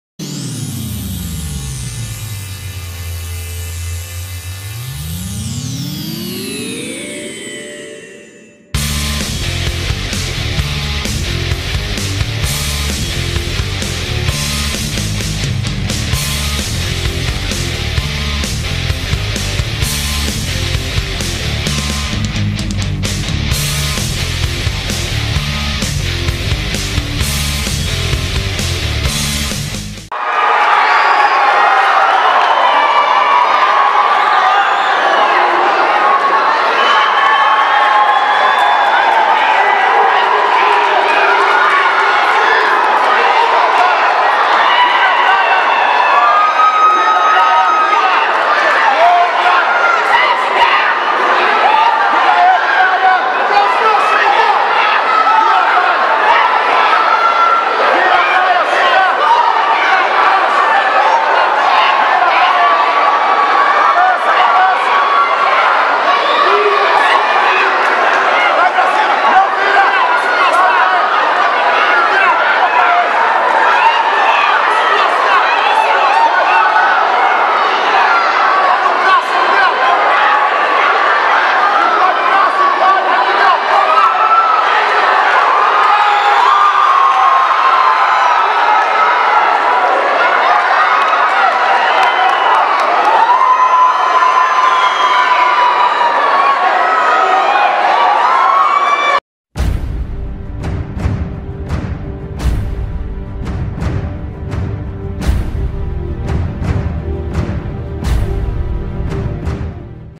Marcos Brayan vs Miguel Amorim - Copa Desafio Bravos
Miguel Amorim (Paviones) venceu Marcos Brayan (Bravo Team).